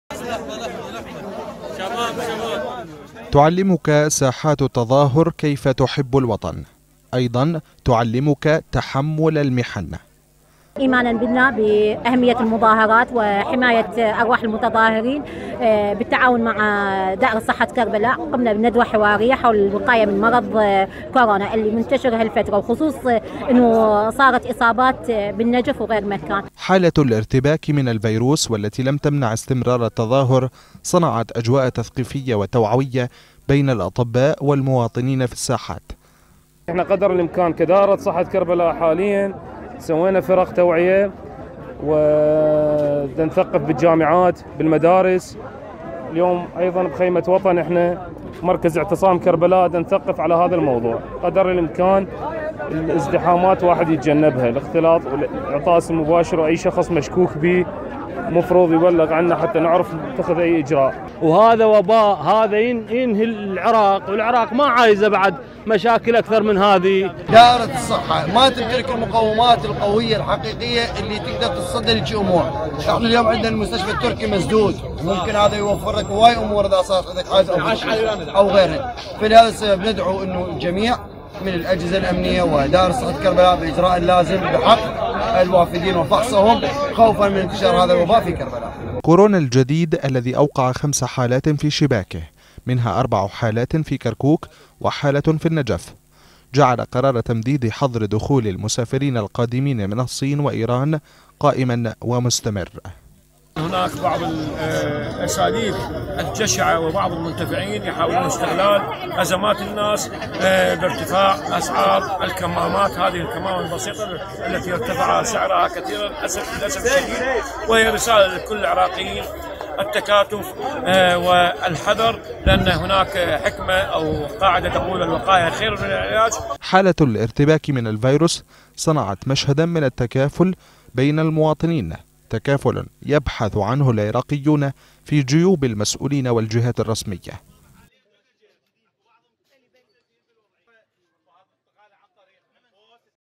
هكذا يواجه متظاهرو كربلاء فيروس كورونا | تقرير
خيمة وطن التي تحتضن متظاهرين في الساحات. تترجم التكافل الشعبي والخوف على العراق من خلال توزيع كمامات الوقاية من كورونا من خلال مبادرة طبية وتثقيفية.